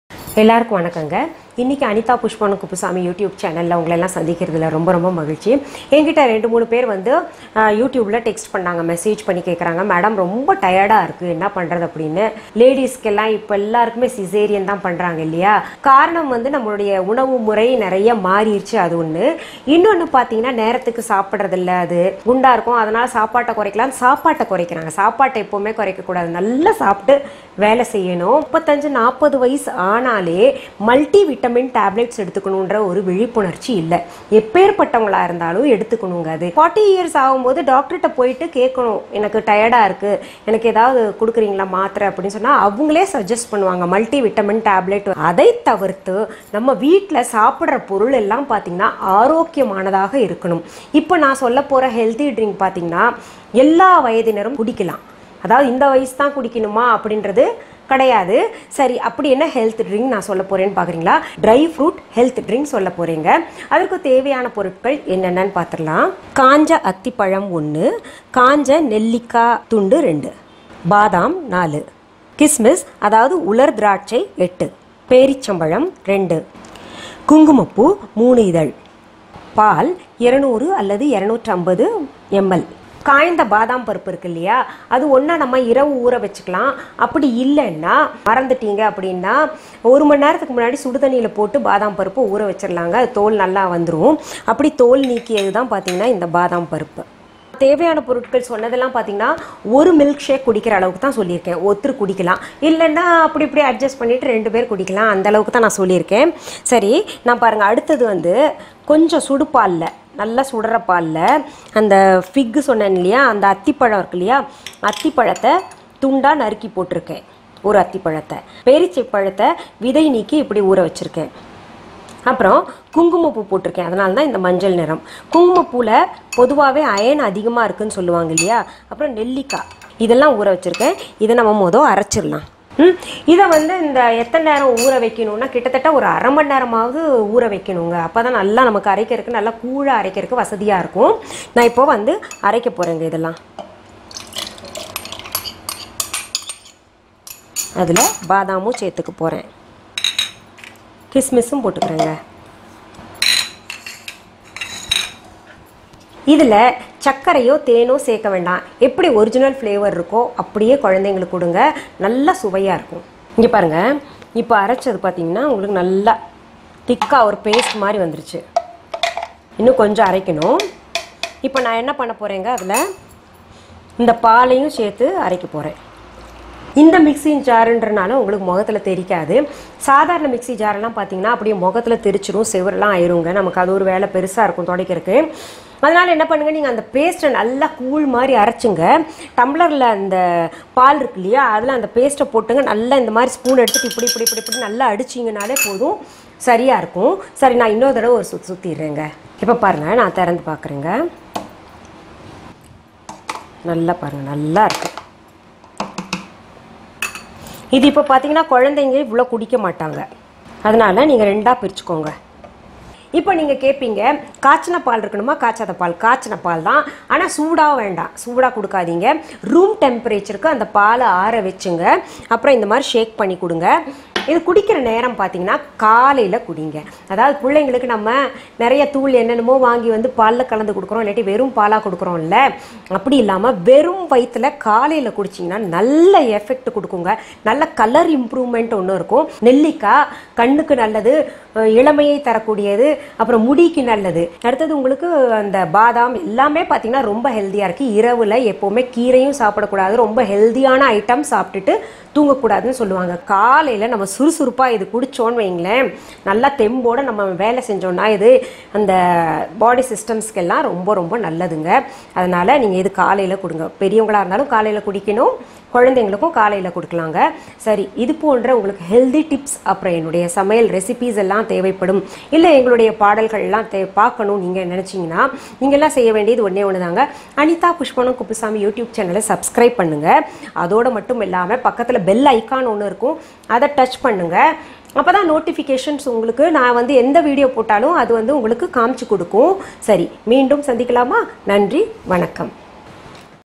சக்தி தரும் பானம் செய்வது எப்படி ? | Home made Healthy Drink mix | Anitha Kuppusamy Health drink
சக்தி தரும் பானம்  செய்வது எப்படி ? என்பதை இந்த வீடியோ பதிவில் அனிதா குப்புசாமி செய்து காட்டியுள்ளார்.

 Home made Healthy Drink mix | Anitha Kuppusamy Health